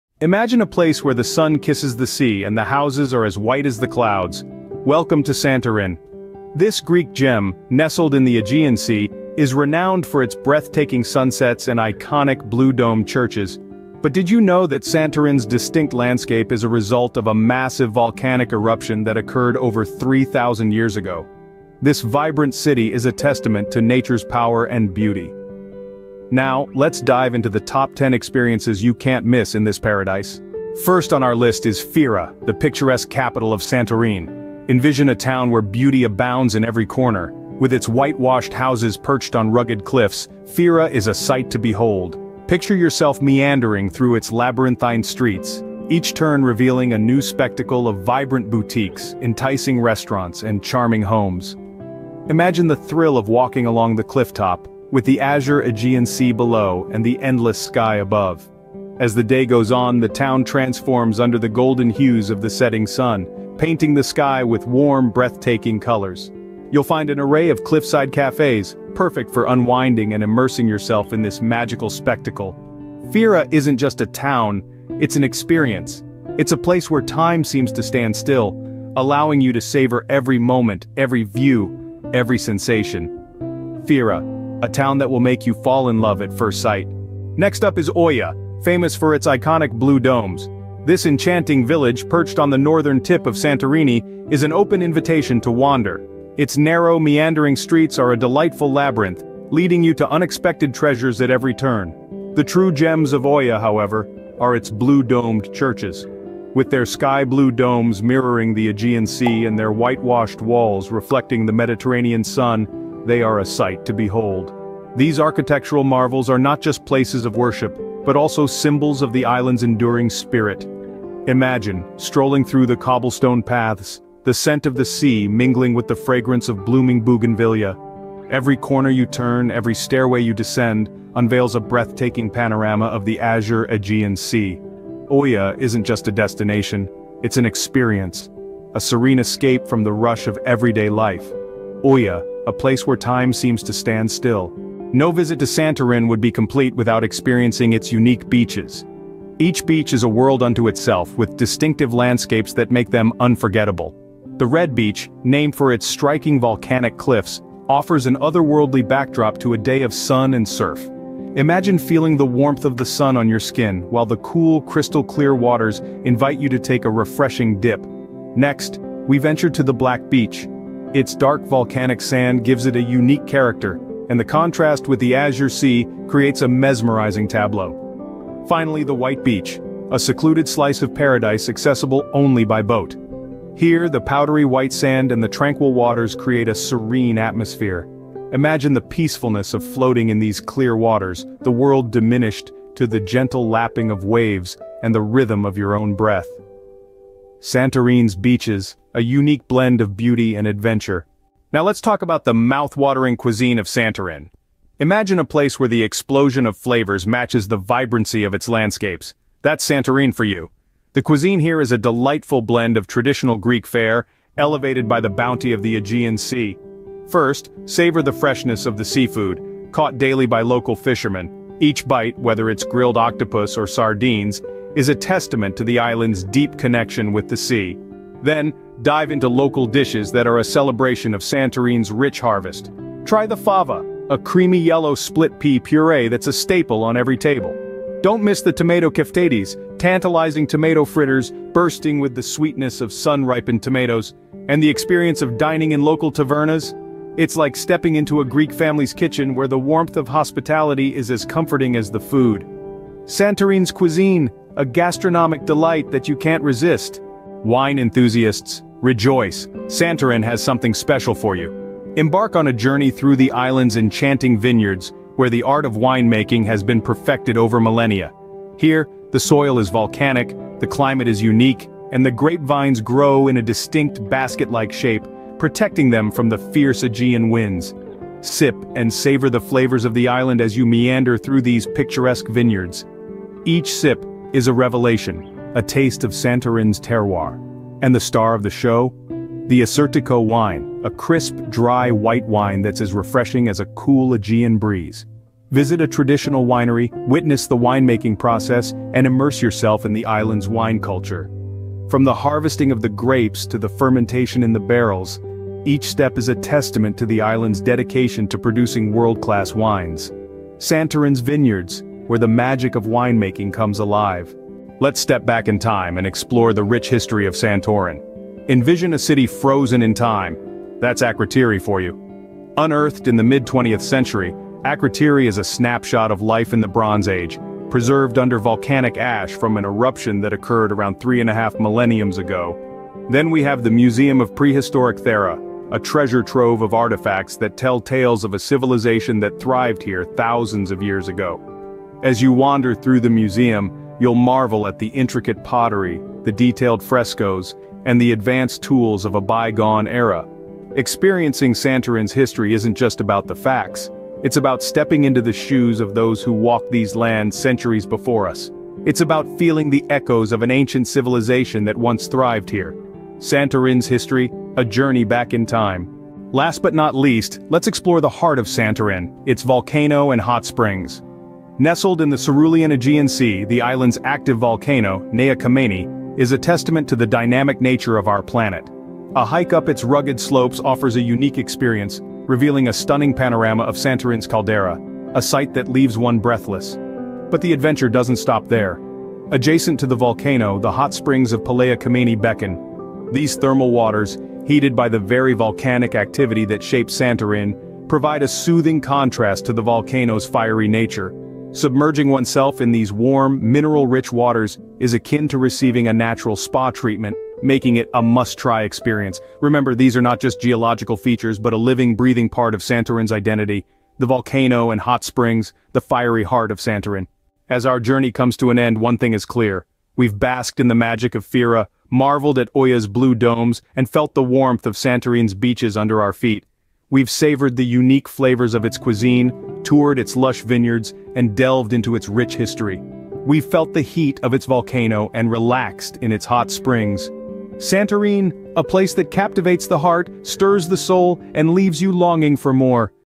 Santorini Greece Travel Guide
🎥 Join us on a journey through Santorini, Greece!  🌃

Come along as we uncover the top 10 unforgettable experiences this stunning island has to offer! Whether you're a local or a first-time visitor, don't miss out! 🇬🇷✨

00:00:00 Welcome to Santorin
00:00:30 The Magic of Fira
00:01:27 The Blue Domes of Oia
00:02:24 The Beaches of Santorin
00:03:21 The Cuisine of Santorin
00:04:17 The Vineyards of Santorin
00:05:14 The History of Santorin
00:06:06 The Volcano and Hot Springs
00:06:57 Farewell Santorin